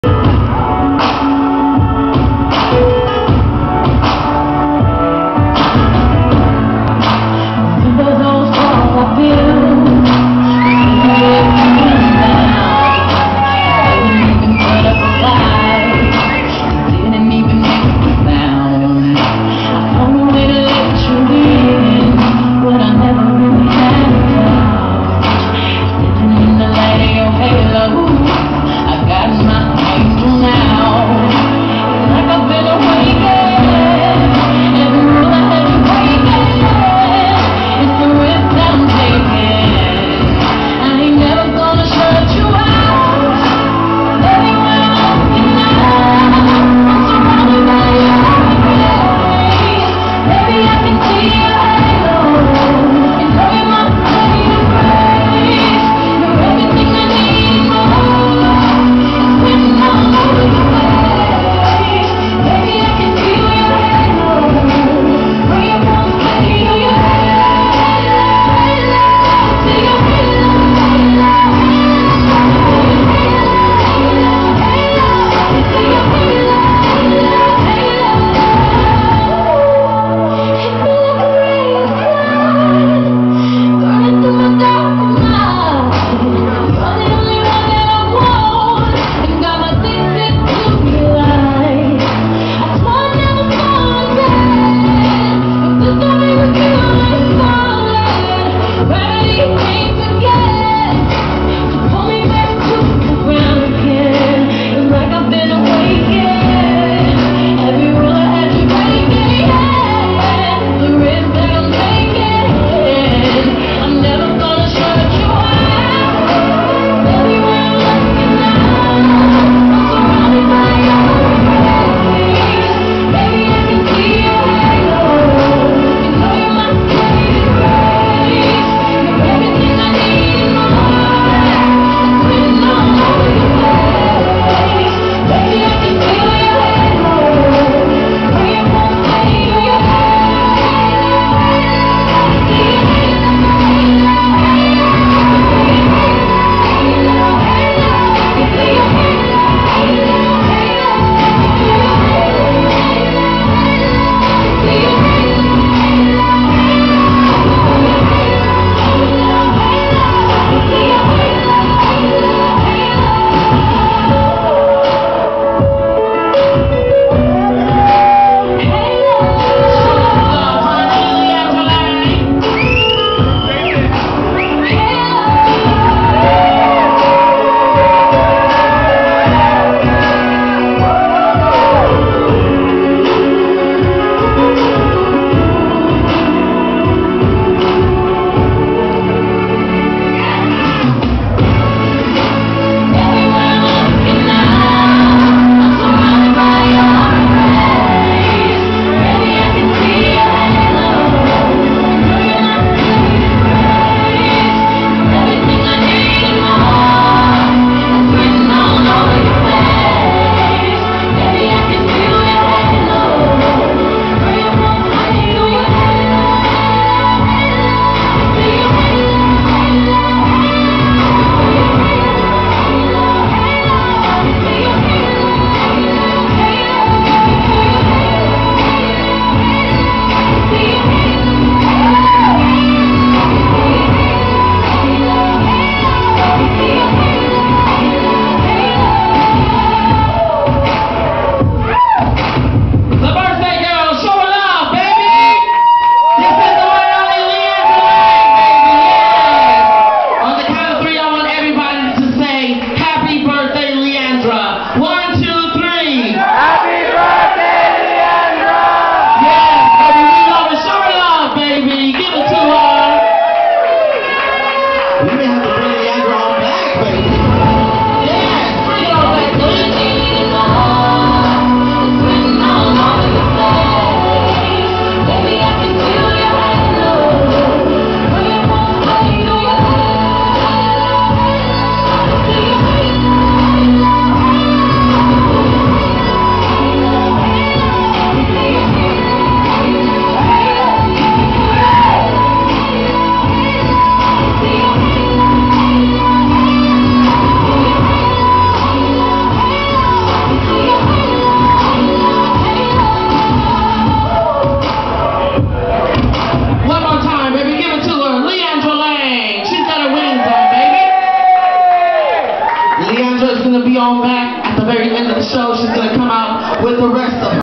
Leandra Langs B-day show 2009
my Beautiful Sister performing  at the heat on her birthday, aint she pretty